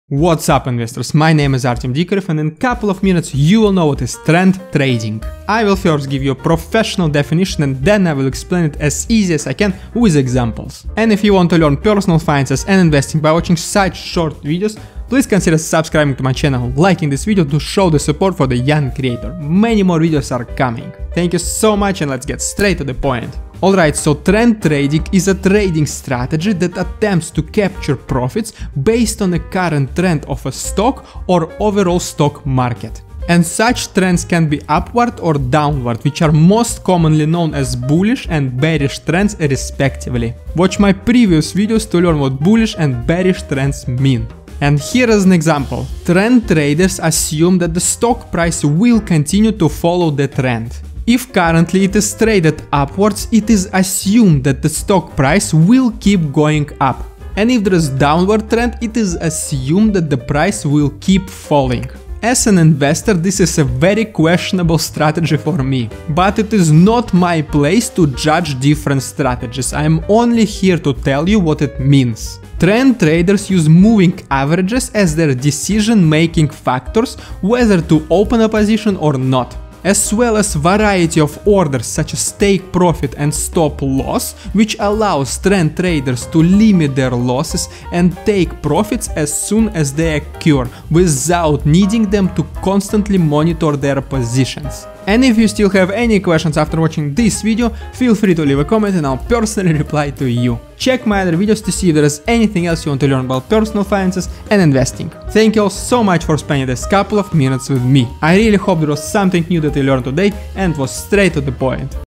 WHAT IS TREND TRADING? Straight to the Point STTP #42
In around two minutes you will know what is Trend Trading. You will get both professional definition and easy explanation. No intro, no outro, straight to the point.

Trend Trading is a trading strategy that attempts to capture profits based on the current trend of the stock or overall market. And such trends can be upward or downward. More commonly known as bullish trend and bearish trend respectively. Watch my previous videos to learn what bullish and bearish trends mean.

Trend Traders assume, that the stock price will continue following the trend. If currently it is trending upwards, it is assumed that the price keeps going up. And if there is downward trend, the price will keep falling.

As an investor, that is a very questionable strategy for me. But it is not my place to judge different strategies. I am here only to tell you what it means.

Trend traders use moving averages as their decision-making factor, whether to enter the position or not. As well as variety of orders, such as take profit and stop loss, that allow trend traders to limit losses, and realize profits as soon as they occur without needing to constantly monitor all of their opened positions.

It helps a lot as a beginner creator. And let me know if there is anything you would like to know about personal finances and investing.